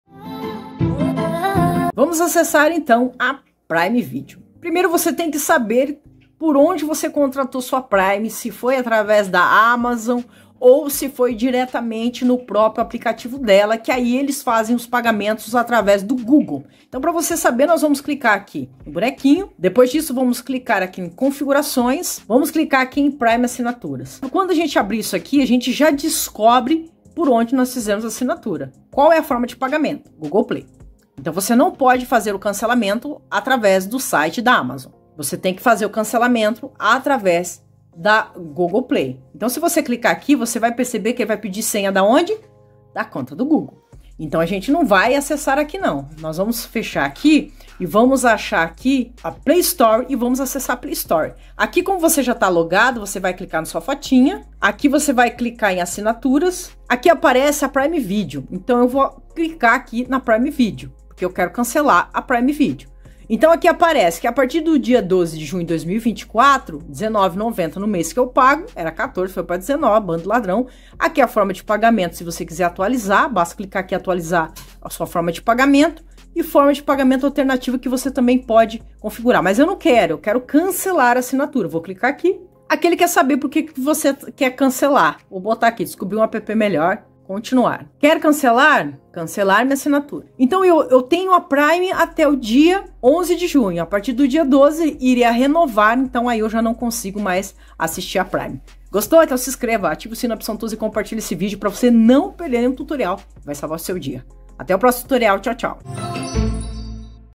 COMO CANCELAR ASSINATURA AMAZON PRIME PELO CELULAR
Está pensando em cancelar sua assinatura do Amazon Prime Video? Neste vídeo, mostramos um passo a passo simples e rápido para cancelar o serviço diretamente pelo seu celular. Siga nossas instruções e resolva isso em poucos minutos!